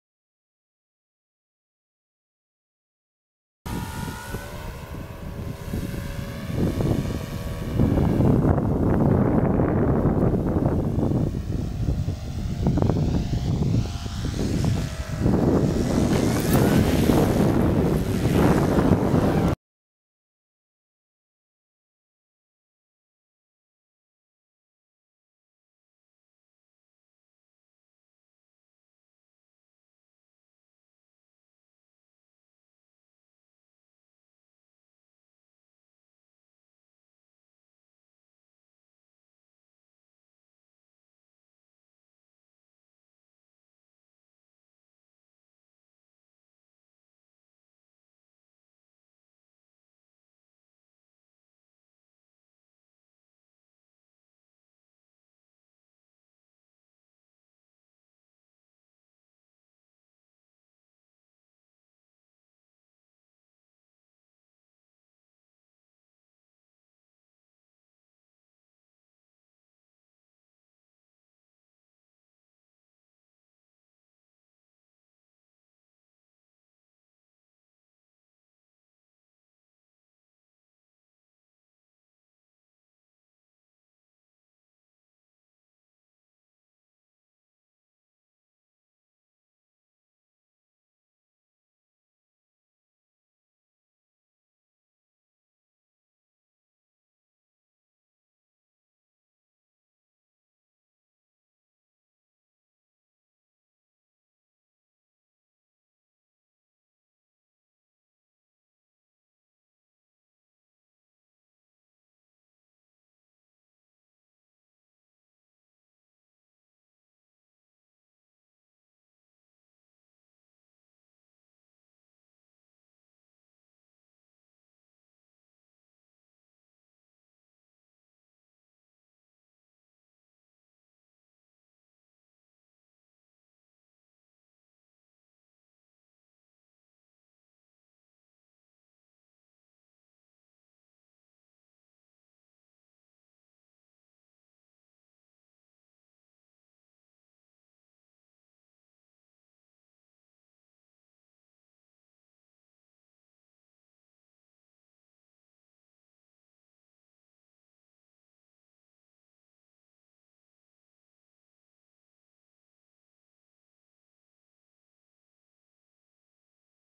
CountSoar and the J3 Kitten.wmv
Our 1 year old Standard Poodle Count got a kick out of chasing the 112" elecric powered J3 Tiger Kitten scale replica of the Home Built full size plane.
Video by Tony Utley (my best frend in our club and a great guy in general, very talented photographer too, he has a wonderful son by the name of Graham whom was present at the filming of this video), Louisville Kentucky. Louisville Soaring Society field, Charlie Vettner Park. 2010.